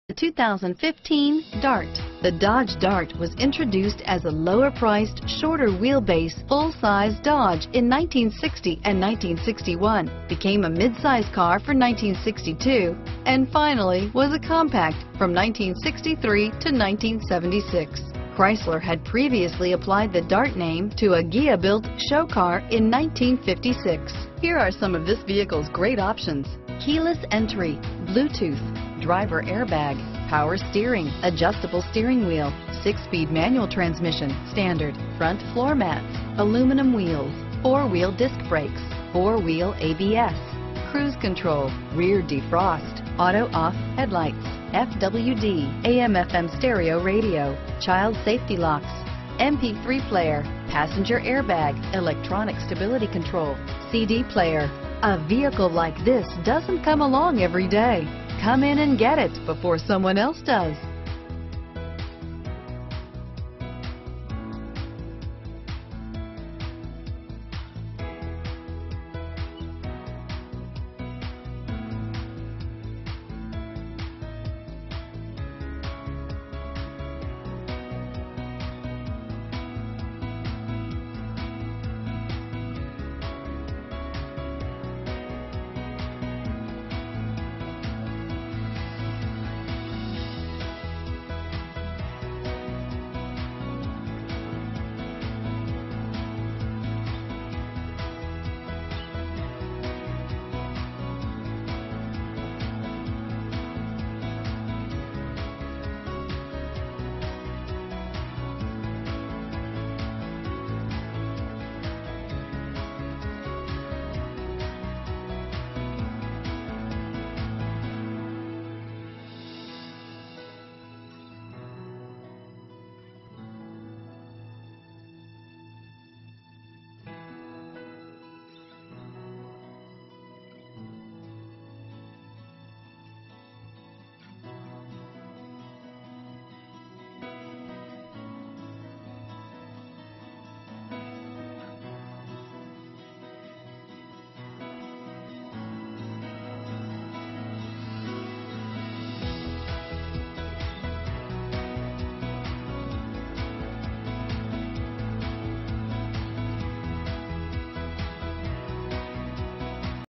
2015 Dodge Dart Kilgore, Henderson, Carthage, Longview, Tyler, TX 336949
2015 Dodge Dart
2015 Dodge Dart 4dr Sdn SXT - Stock#: 336949 - VIN#: 1C3CDFBB0FD336949

FUEL EFFICIENT 35 MPG Hwy/22 MPG City! SXT trim. CD Player, Head Airbag, Aluminum Wheels, Bluetooth, iPod/MP3 Input. CLICK ME! KEY FEATURES INCLUDE: iPod/MP3 Input, Bluetooth, CD Player, Aluminum Wheels. MP3 Player, Remote Trunk Release, Keyless Entry, Child Safety Locks, Steering Wheel Controls. VEHICLE REVIEWS: Great Gas Mileage: 35 MPG Hwy. Fuel economy calculations based on original manufacturer data for trim engine configuration. Please confirm the accuracy of the included equipment by calling us prior to purchase.